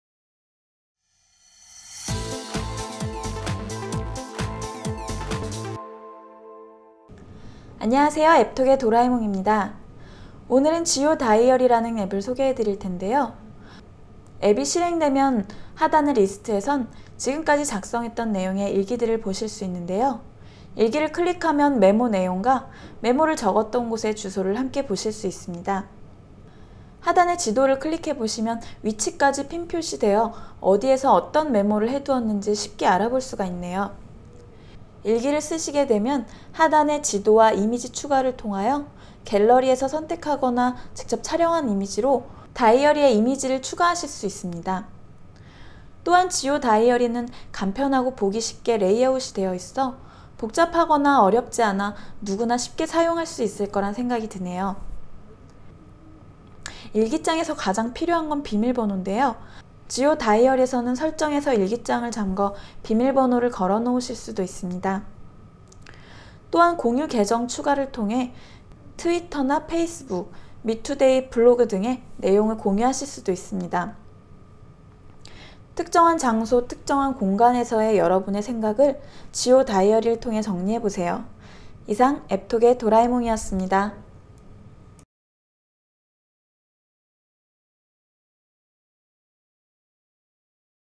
geo diary_galaxys_android.
안드로이드폰에서 메모, 사진, 지도를 첨부할 수 있는 쉬운 다이어리 앱!
오시면 더 많은 영상 리뷰를 보실 수 있습니다 :3